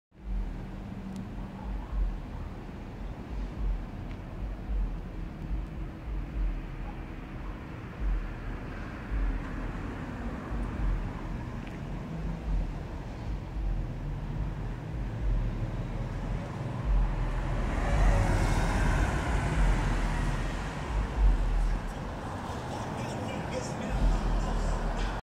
Porsche G50 911 Targa Exhaust
Filmed on 6/8/17, utilizing a HTC One M7. A BMW F82 M4 speeds by as I head in. I noticed the Porsche 911 G50 Targa. Plus over the right lane is a Range Rover Land Rover. Share on Tumblr, Pinterest, Porsche sites.